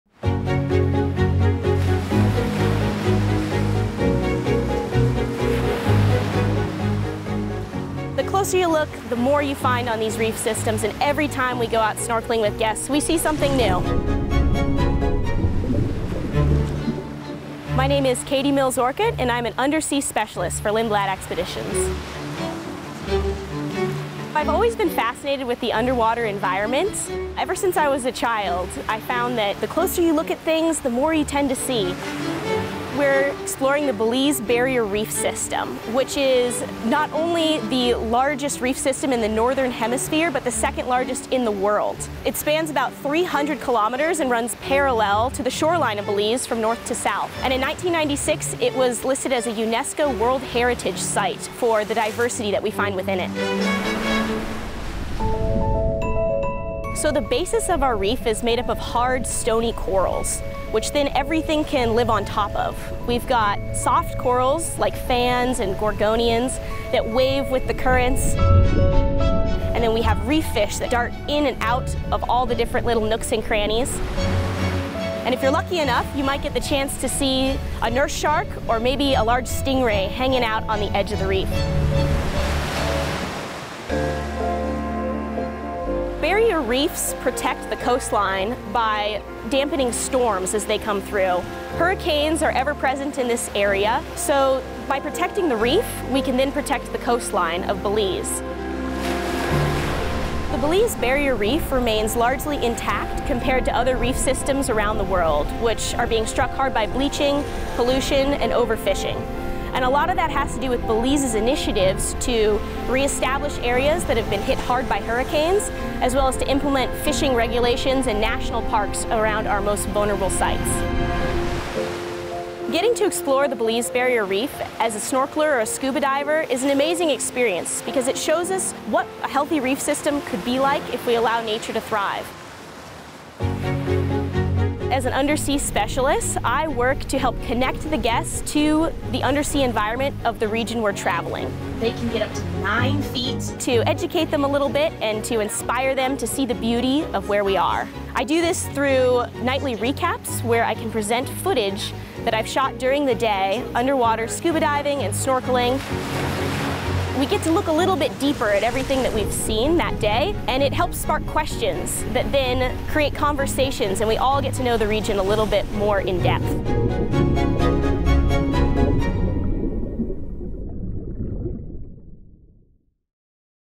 Exploring the Belize Barrier Reef | Expedition Spotlight | Lindblad Expeditions
Visiting the Belize Barrier Reef is a highlight of our expeditions in Belize and Guatemala. In the latest episode of Expedition Spotlight, Lindblad Expeditions undersea specialist Katie Mills Orcutt discusses this thriving underwater ecosystem, the creatures who live there, and how our guests experience it all through snorkeling and zodiac cruising.